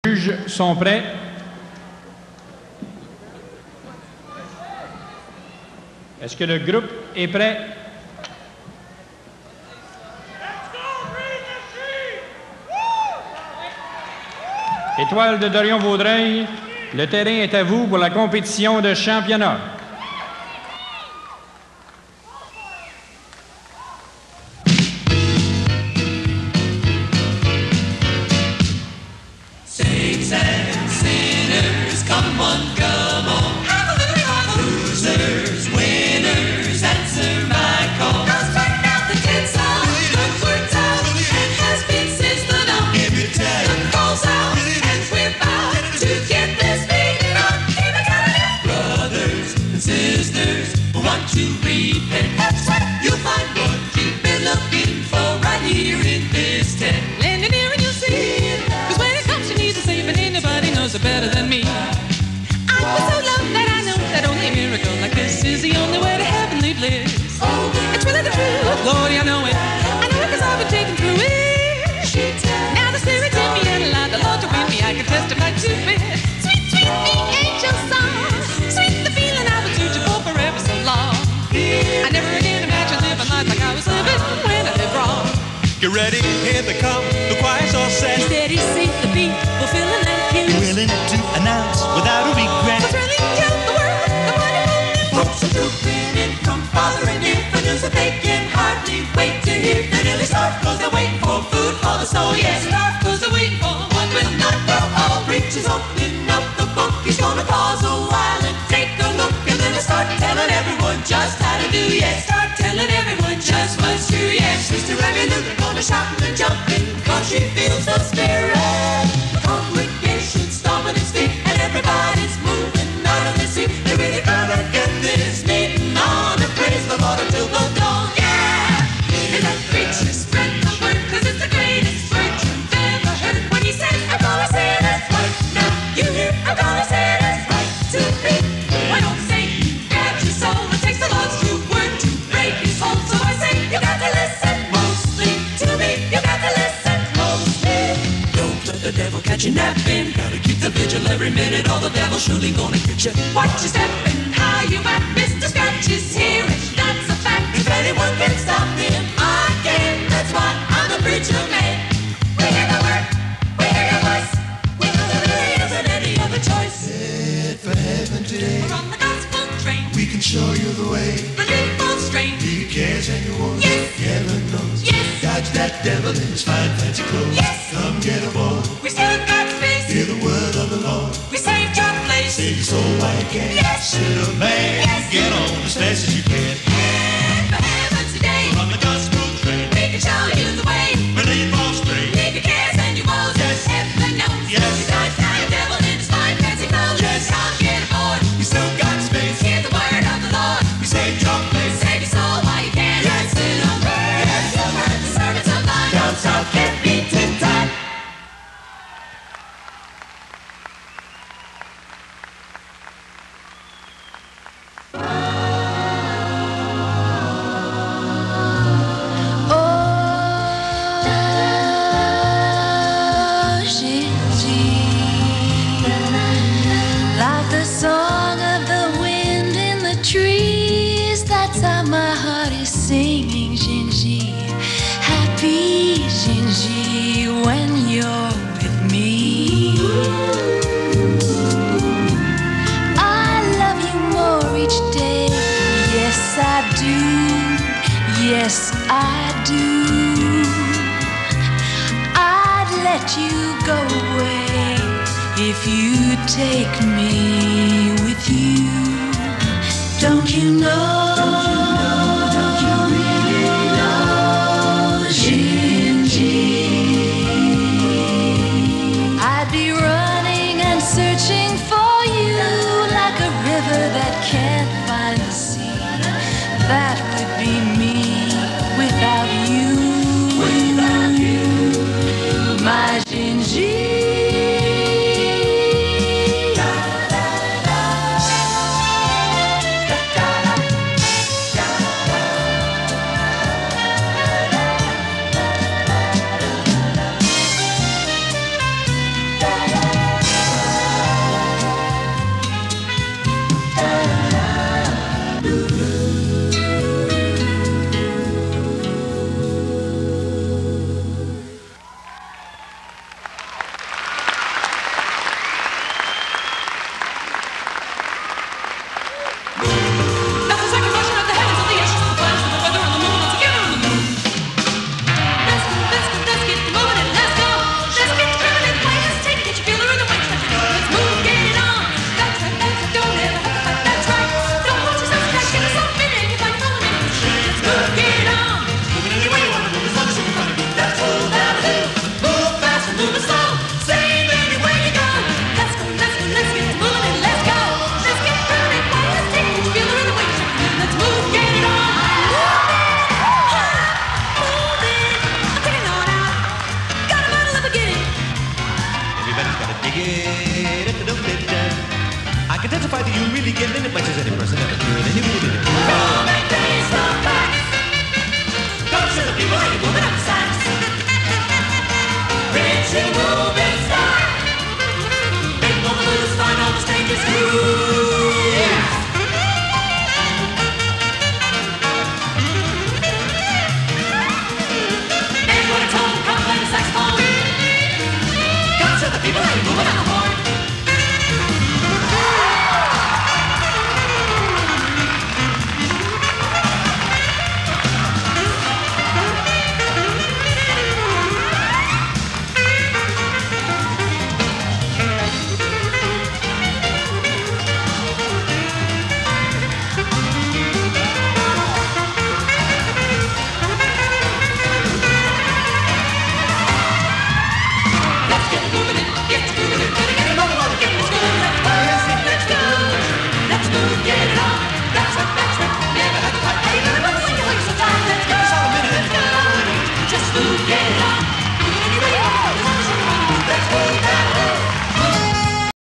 Les Étoiles IO 1986 Guard (CECQ Finals)
Les Étoiles from Dorion-Vaudreuil, Québec, Canada
Independent Open class
CECQ Provincial Championship Finals 1986
Victoriaville, Québec, Canada